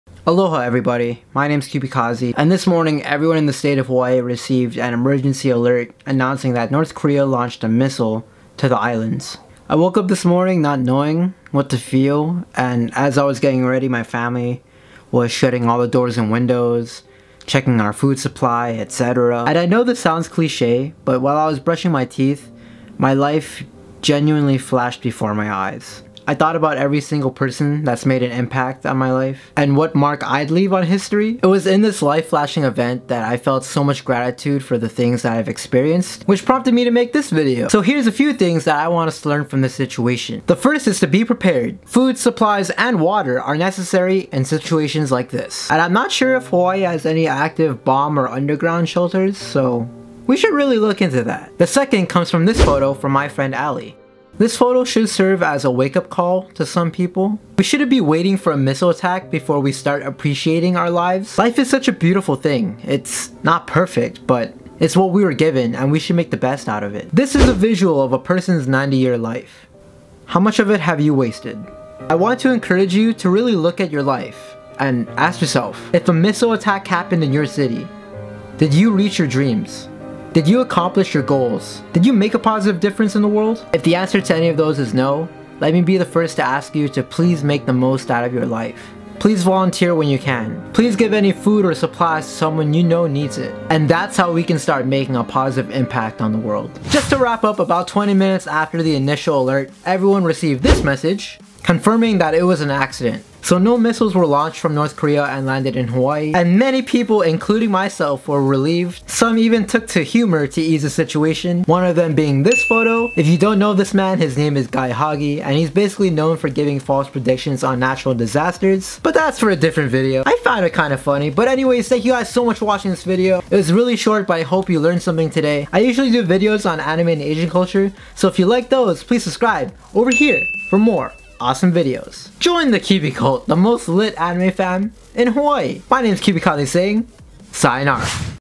Hawaii got a Missile Threat from North Korea... "THIS IS NOT A DRILL"
Hawaii got a Missile Threat from North Korea... who woulda thought?

Like really, I honestly had high hopes for resolution, until this morning. I woke up to an emergency alert and immediately got out of bed to see all my friends and family talking about it. When I was brushing my teeth, I got an epiphany of some sort. I'm an optimistic person, and I knew me, my friends, and family would be safe, but subtle fear still lingered in my mind. I started looking back at my life experiences which prompted me to make this video. Whatever happens in the near future, I pray you and your loved ones are all safe and enjoy life to the fullest!